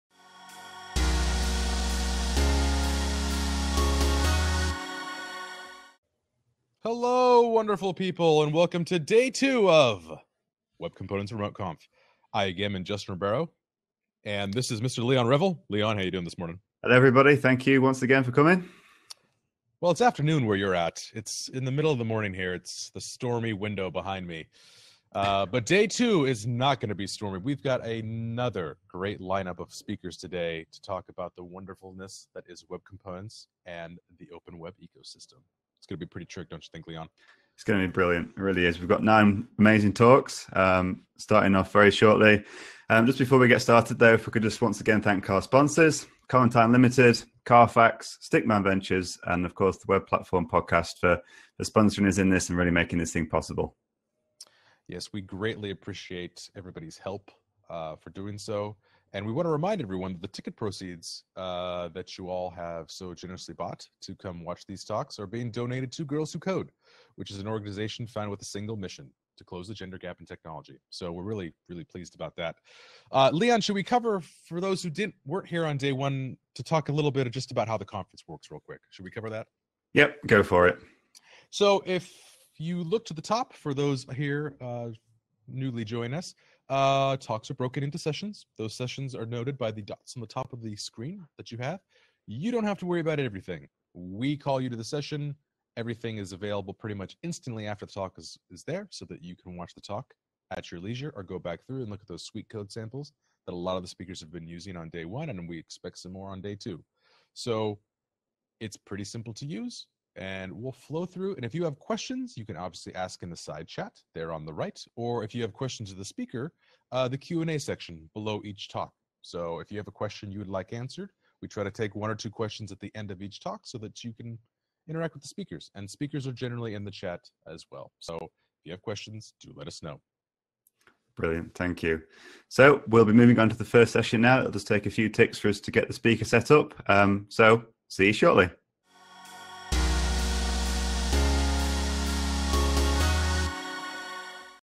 Opening Day 2 with Justin Ribeiro and Leon Revill | WCRC 2017
Kicking off part 2 of Web Components Remote Conference!

Speakers: Justin Ribeiro (@justinribeiro) and Leon Revill (@RevillWeb)

Follow @wcremoteconf for updates and info on next years conference!